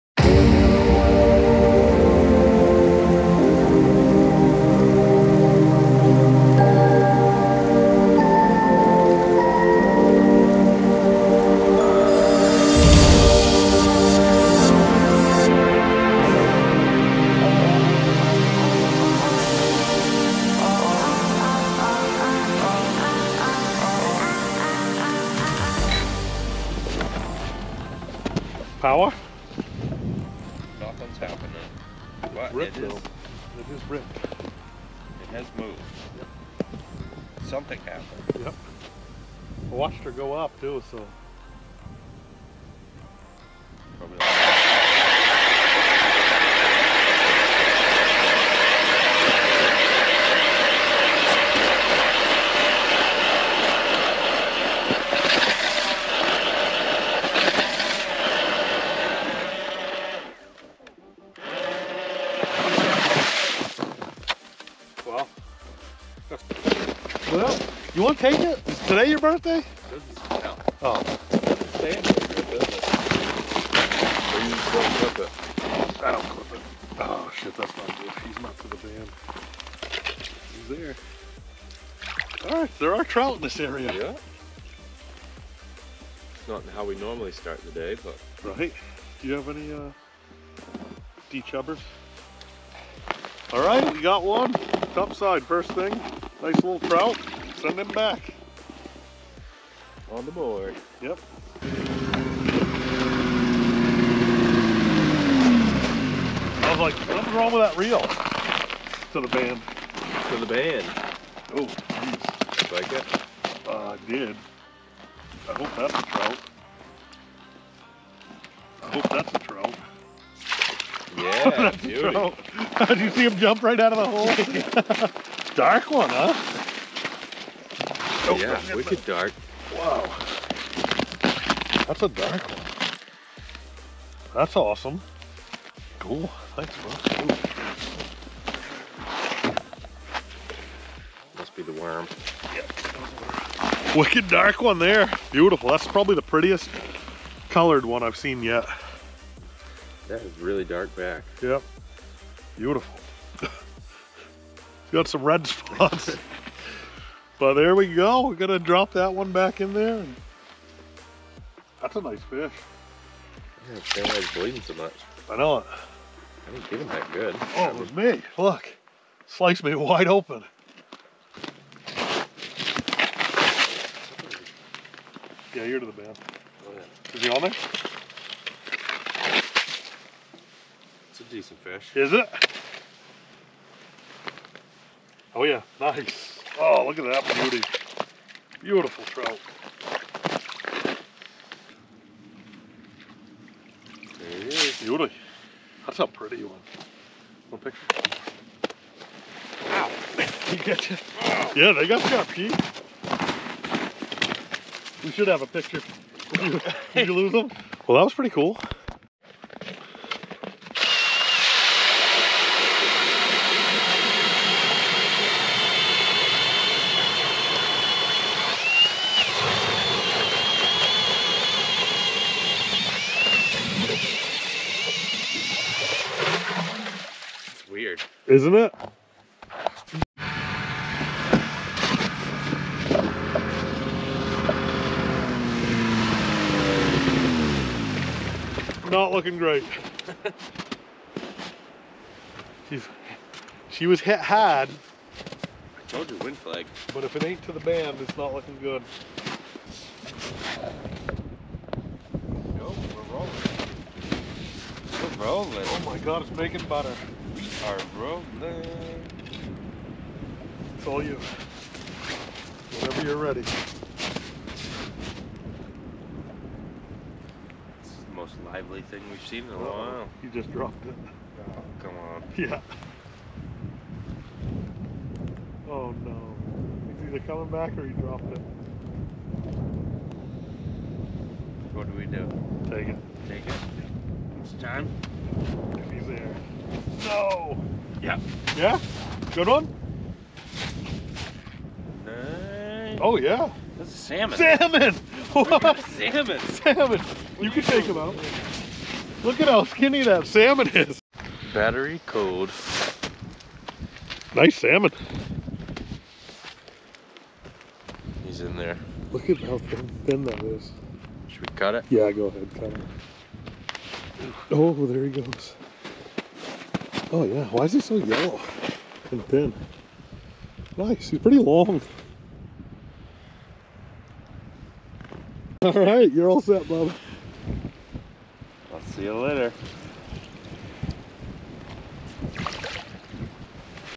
5 Minutes Ice Fishing 4 Native Brook Trout
A quick little video, 5 minutes of fishing Moosehead Lake with a big surprise at the end. Check out how deep the ice got on this shore at the end of last season. Super pretty brook trout. Shawn and I get out of the weather and check out a new spot to us on the lake!
Targeting the illusive native Maine Moosehead Brook Trout.